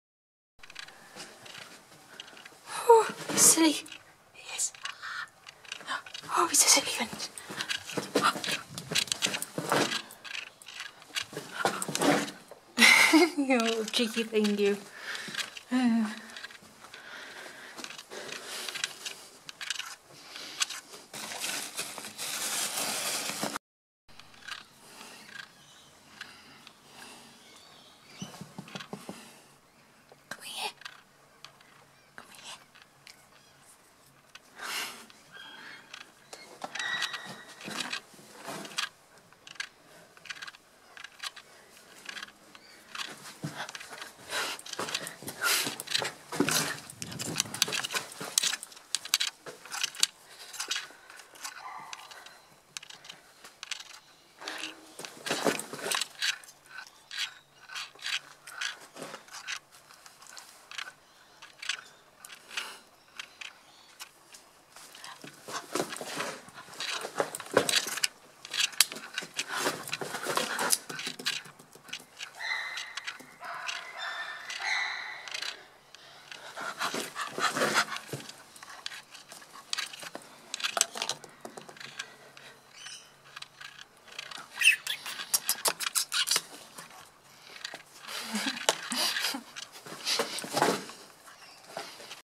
Playing with Winton (bonobo.)
Winton is a little scamp.
Ugh, I'm so sorry you have to put up with my obnoxious voice. And yes I'm doing bonobo laugh impressions even though he probably can't hear me through all that double/triple glazing.